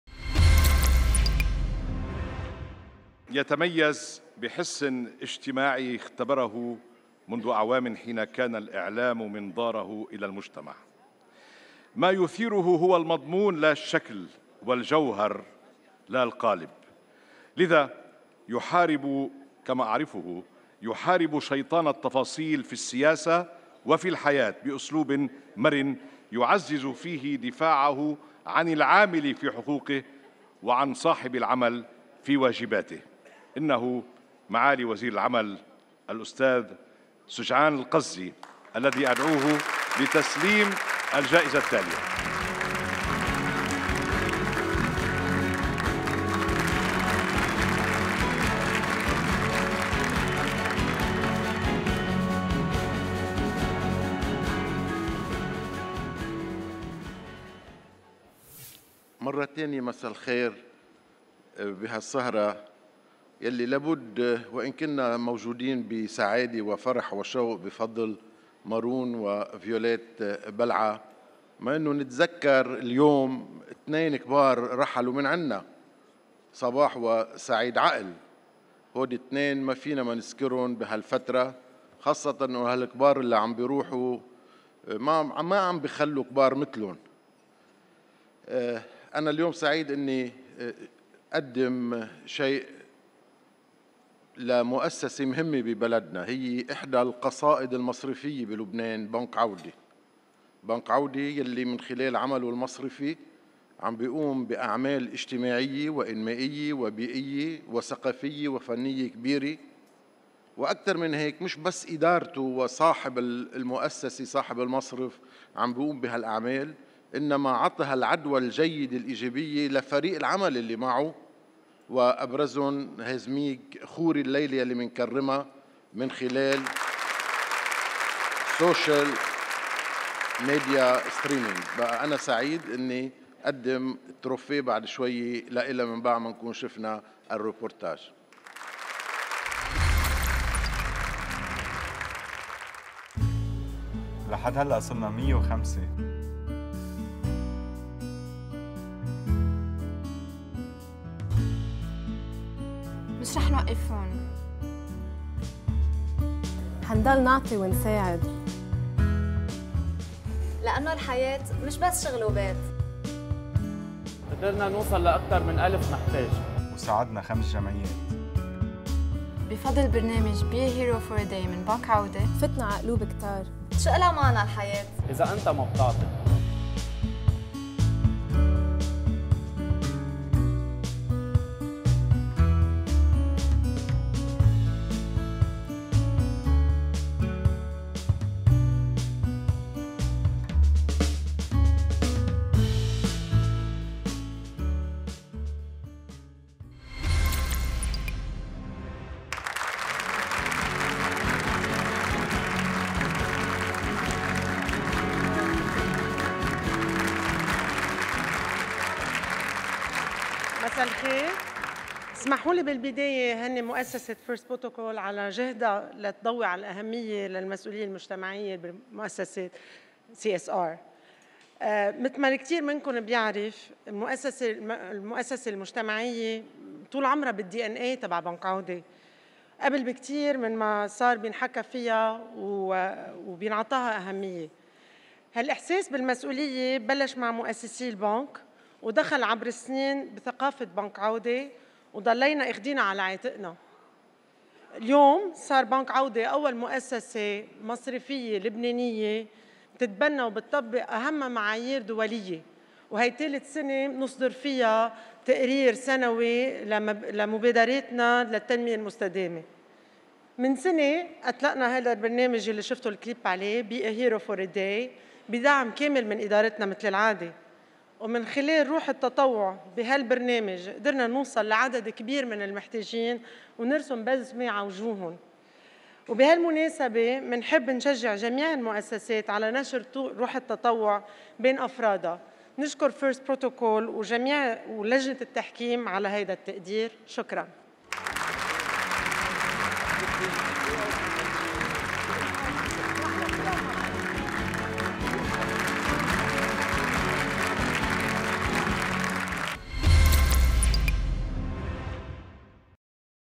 Bank Audi, Winner SEA 2014, Social Main Streaming
H.E. Mr. Sejaan Azzi honoring Bank Audi, represented by Mrs. Hazmig Khoury, for the Social Main Streaming Category